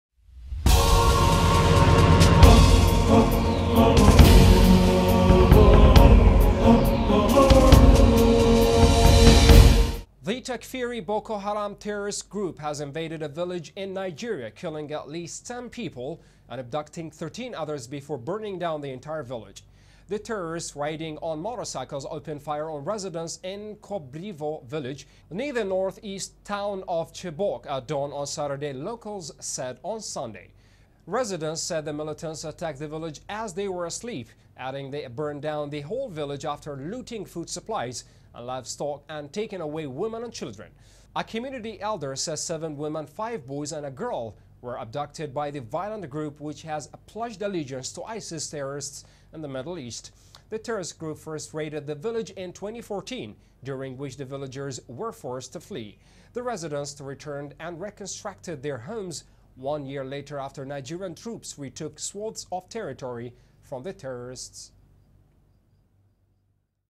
Boko Haram militants kill 10, abduct 13 in Nigerian village .2016/08/24
The Takfiri Boko Haram terrorist group has invaded a village in Nigeria, killing at least ten people and abducting 13 others before burning down the entire village.
The terrorists riding on motorcycles opened fire on residents in Kubrrivu village near the northeast town of Chibok at dawn on Saturday, locals said on Sunday.
Residents said the militants attacked the village as residents were asleep, adding they burnt down the whole village after looting food supplies and livestock and taking away women and children.
A community elder said seven women, five boys and a girl were abducted by the violent group which has pledged allegiance to ISIS terrorists in the Middle East.
The terrorist group first raided the village in 2014, during which the villagers were forced to flee. The residents returned and reconstructed their homes one year later after Nigerian troops retook swathes of territory from the terrorists.